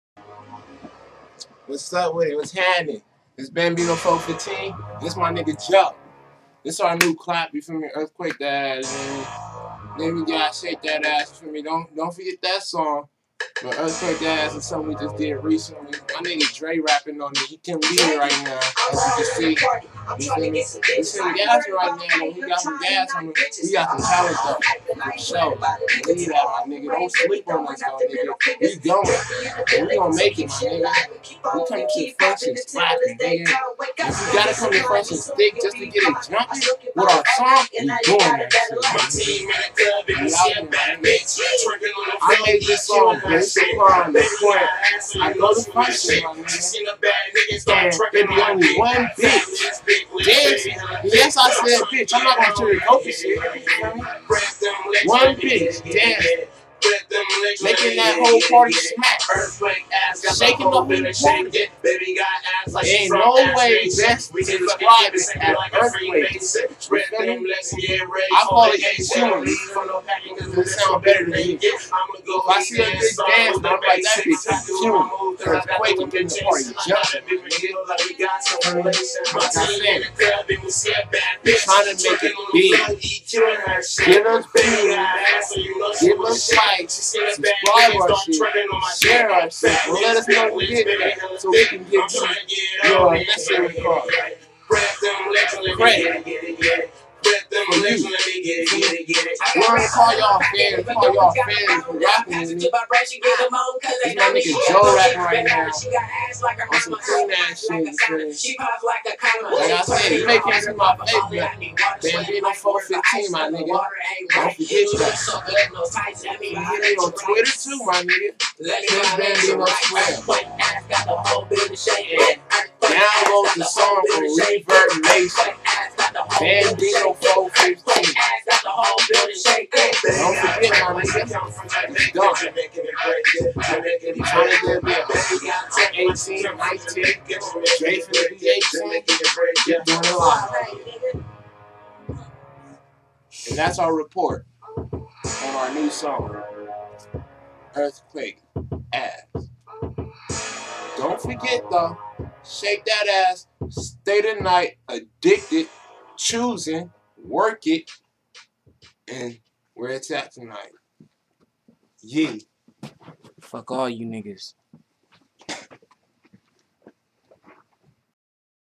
Ep.1 F.o.C News Report:Earthquake Ass
Me and my nigga Slient Joe aka Jojo suave fuckn around with the Web cam reporting F.o.C news fo the first time Warning: this is a rough draft if your under the age of 18 please have guardian parent with you.Naww im juss fuckn around lmao